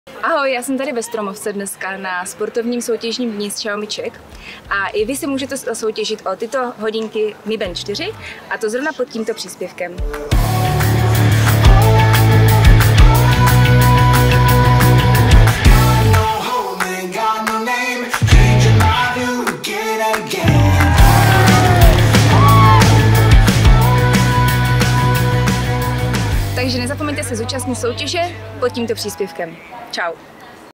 Soutěž s Karolínou Huvarovou o nový Mi Band 4 | Xiaomi Czech
VELKÁ PRÁZDNINOVÁ SPORTOVNÍ SOUTĚŽ!
Soutěžte s námi na našem Instagramovém profilu o žhavou novinku - 10x chytrý náramek Mi Band 4 pod tímto příspěvkem

Pravidla jsou jednoduchá:
1) Sleduj Instagram stránku @XiaomiCzech
2) Dej "LIKE" na toto video.
3) Napiš do komentářů pod soutěžní video na Instagram profilu @XiaomiCzech, kterému týmu jsi fandil právě ty!

Soutěž vyhlásíme na našem Instagramovém účtu @XiaomiCzech a to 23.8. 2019, kdy vybereme 10 výherců chytrého náramku Mi Band 4.

Podpoř také své oblíbené influencery hashtagy XiaomiOrange nebo XiaomiWhite na vlastních sociálních sítích.

Přejeme všem hodně štěstí.